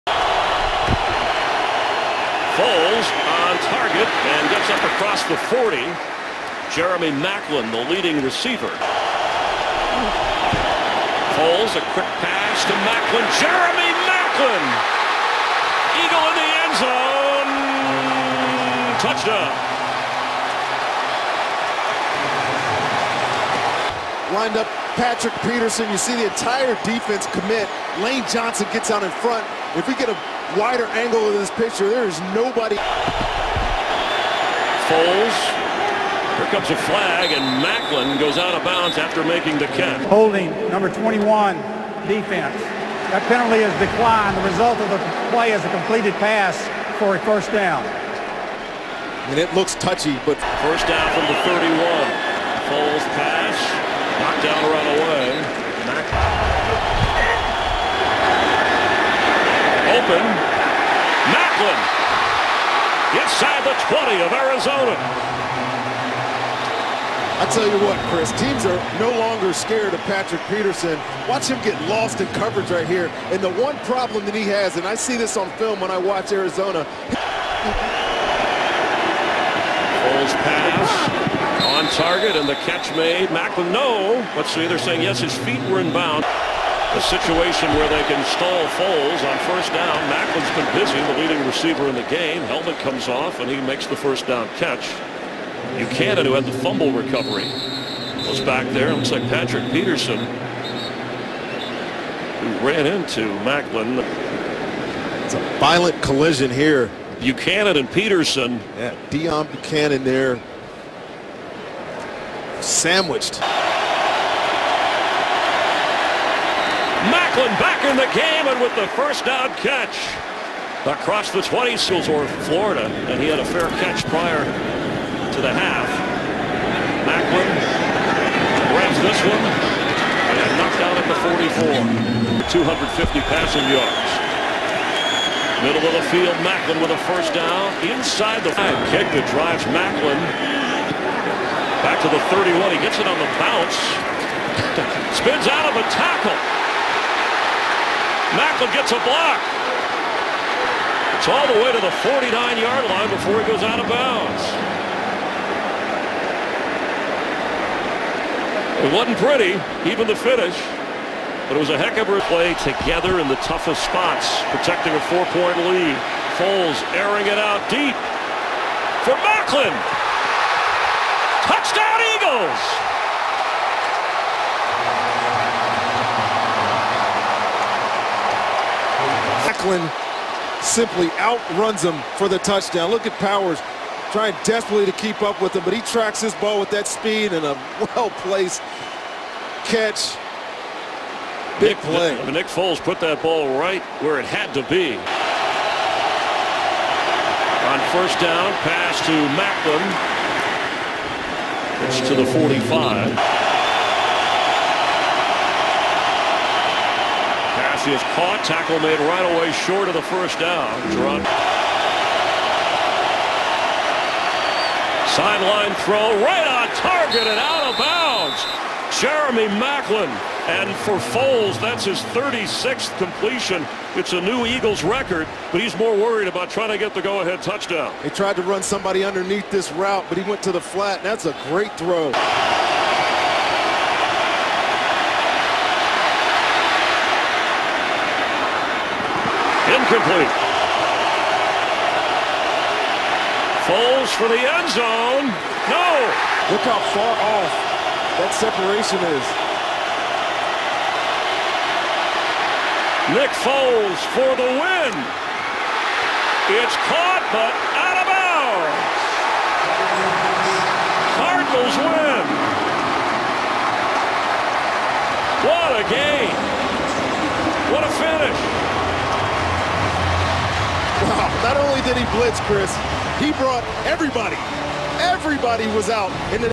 The Game Jeremy Maclin DESTROYED Patrick Peterson | Throwback Highlights 10.26.2014
One of if not the best game of Jeremy Maclin career against one of the best corners in the game in Patrick Peterson.

STATS: 12REC 187YDS 2TDs  2RET 23YDS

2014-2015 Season